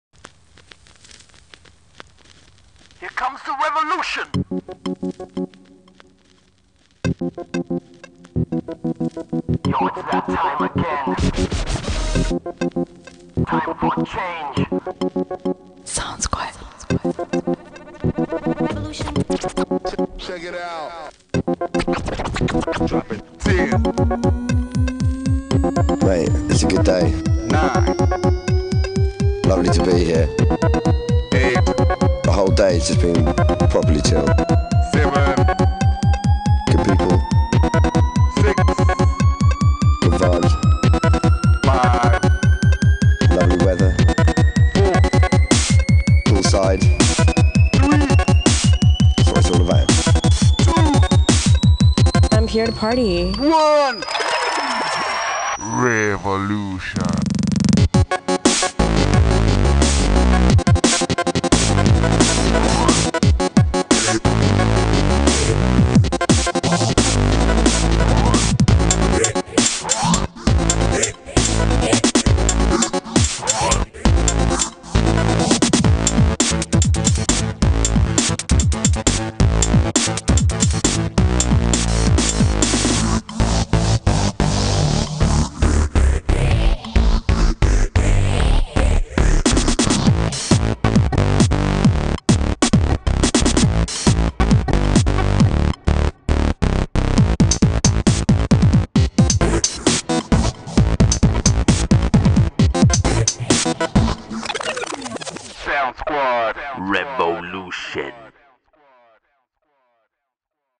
Revolutions 2009
Sound Squad "Revolutions" Outdoor Electronic Music Festival - South Africa, Cape Town

12 December 2009